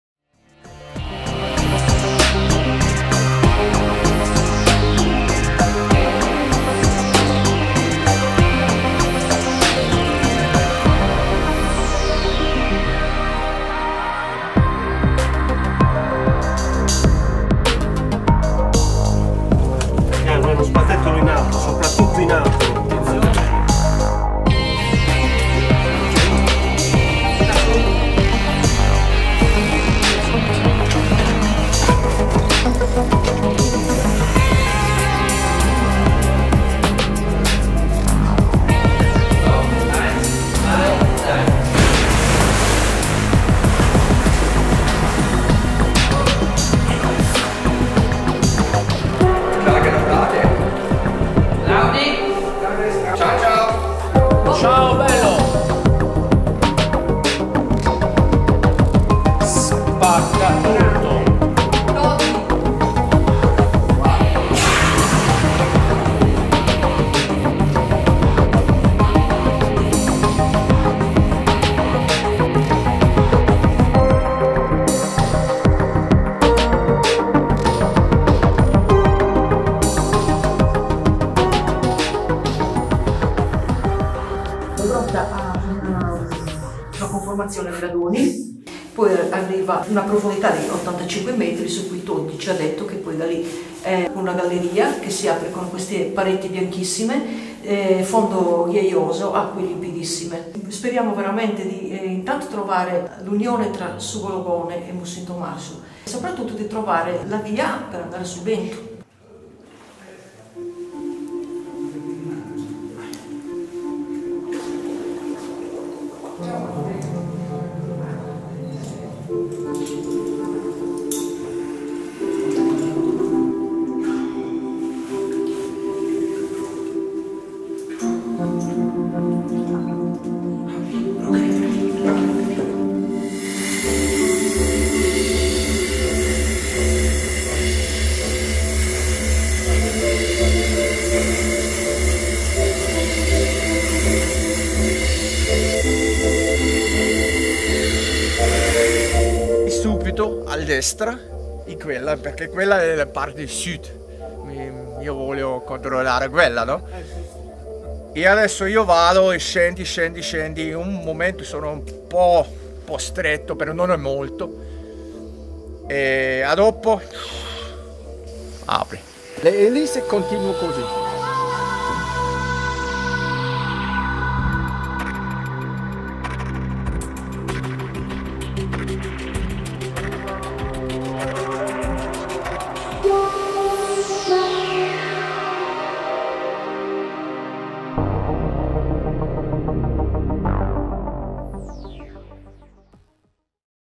Grotta di Mussin Tomasu importanti scoperte speleosubacquee
La grotta di Mussin Tomasu è delle principali risorgenti a nord del massiccio carsico del Supramonte, connessa con la vicinissima risorgente di Su Gologone. All'interno di un progetto di esplorazione della zona satura del grande complesso carsico del Supramonte, lo speleosub Toddy Waelde del Team Protec Sardinia con tecniche avanzate e rebreather si inoltra nei condotti carsici fino alla profondità di 124m, seguendo verso sud l'inizio di una grossa galleria discendente che sembra la condotta principale di alimentazione delle due risorgenti. Un momento storico per le ricerche nel bacino profondo, dopo decenni di immersioni nella vicina risorgente di Su Gologone, con poche possibilità di prosecuzione per via degli stretti passaggi. Le prossime immersioni saranno determinanti per le informazioni e le immagine che arriveranno dalle profondità del Supramonte.